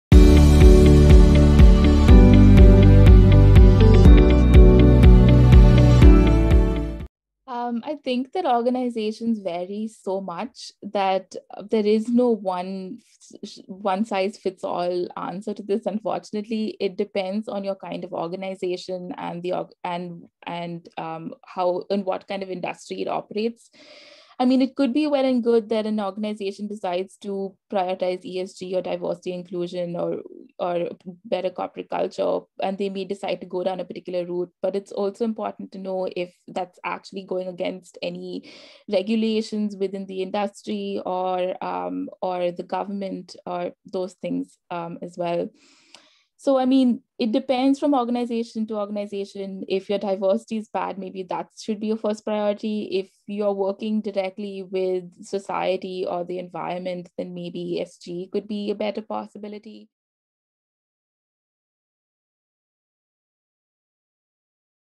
From a millennials perspective what is the subject that is priority for boards?
Summary:

Does the world of corporate governance seem daunting to you? Would you like to know more about it, but just don’t know where to start?This webinar aims to demystify corporate governance by providing you a fresh take on what it really is and how it’s shaping our world today.

Speaker:

This webinar was presented by Rhea Albuquerque who is head of product development at the Corporate Governance Institute.